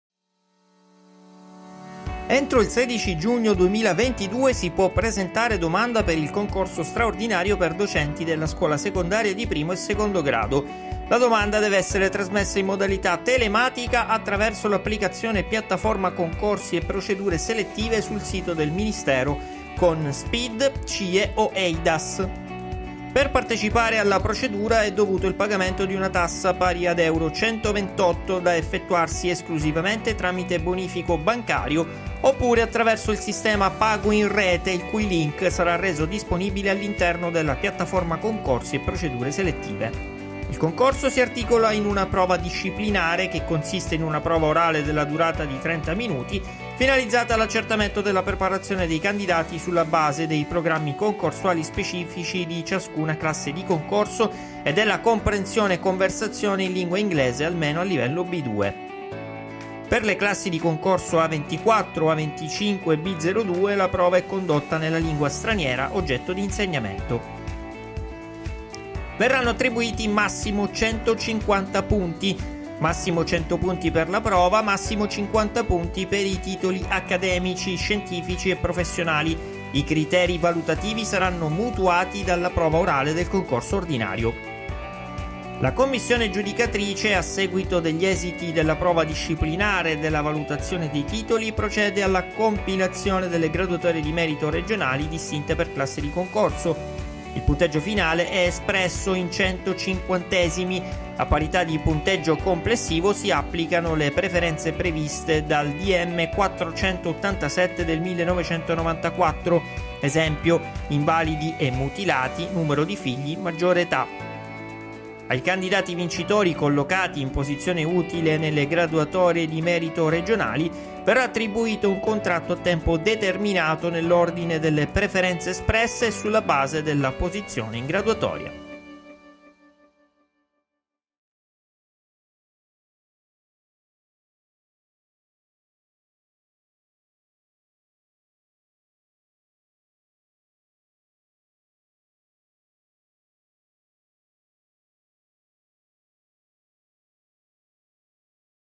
Concorso straordinario secondaria: prova disciplinare e valutazione - VIDEO TUTORIAL
concorsostraordinario tecnicadellascuola docenti Entro il 16 giugno 2022 si può presentare domanda per il concorso straordinario per docenti della scuola secondaria di I e II grado. La domanda deve essere trasmessa in modalità telematica attraverso l’applicazione «Piattaforma concorsi e procedure selettive» sul sito del Ministero (con SPID, CIE o EIDAS).

Al video tutte le indicazioni sulla prova e il punteggio.